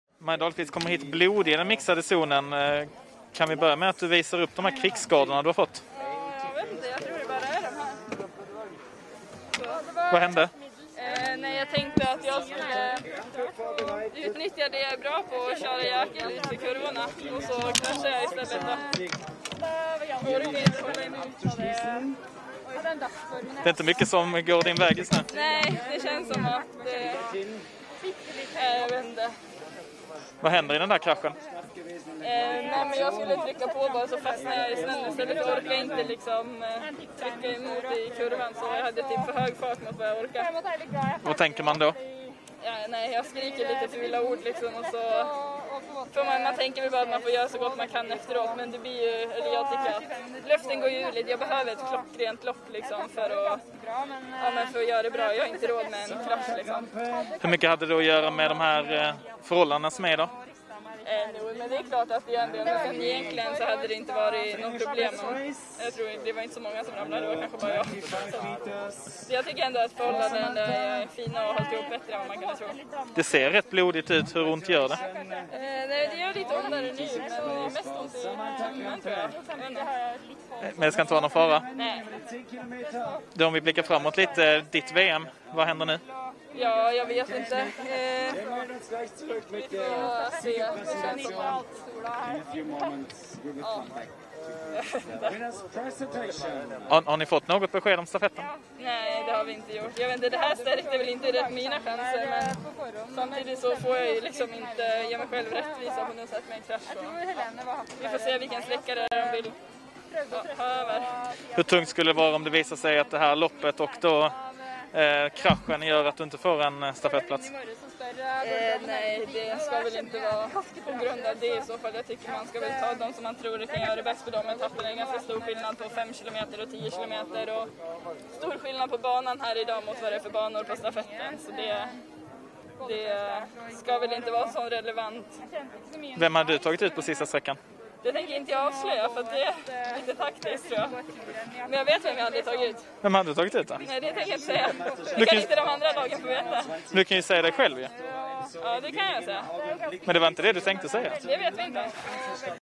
Reportern: Här kommer Maja Dahlqvists blodig...
SKID-VM 20121
OBERSTDORF/STOCKHOLM.
Okända dramat som tv-tittarna inte fick se

Maja Dahlqvist slutade 34:a i 10 kilometer fristil.

Men det tv-tittarna missade var att sprintexperten kraschade och slogs blodig.

– Det gör lite ondare nu, men mest ont i tummen. Men det är ingen fara, säger hon.

Den sista platsen i det svenska laget till 10 kilometer fristil gick till Maja Dahlqvist. Men guldmedaljören i sprintstafetten hade problem när det vankades distans.

Till sist slutande hon 34:a med nästan tre minuter upp till den överlägsna vinnaren Therese Johaug. Och när Dahlqvist gick i mål gjorde hon det med en blodig underarm.

– Jag tänkte att jag skulle utnyttja det är jag är bra på, men så orkar jag inte hålla emot i kurvan så jag kraschar i stället.

– Jag orkar inte trycka emot i kurvan så jag fastnar i snön, förklarar hon i den mixade zonen.
”Jag skriker lite fula ord”

Det är inte mycket som går din väg just nu?

– Nej, jag vet inte.

– Jag skriker lite fula ord och man tänker väl att man ska göra så bra man kan efteråt, men luften går ju ur lite. Jag behöver ju ett klockrent lopp för att göra det bra, jag har inte råd med en krasch.

Oberstdorf har hittills bjudit på strålande solsken och flera plusgrader, arrangören har därför fått kämpa för att få till rättvisa förhållanden. Men Dahlqvist vill inte skylla fallet på underlaget.

– Det är klart att det gör en del men sen så tror jag inte att det gjorde så mycket. Det var inte så många som ramlade, det var kanske bara jag. Så att jag tycker ändå att förhållandena har hållit ihop rätt bra.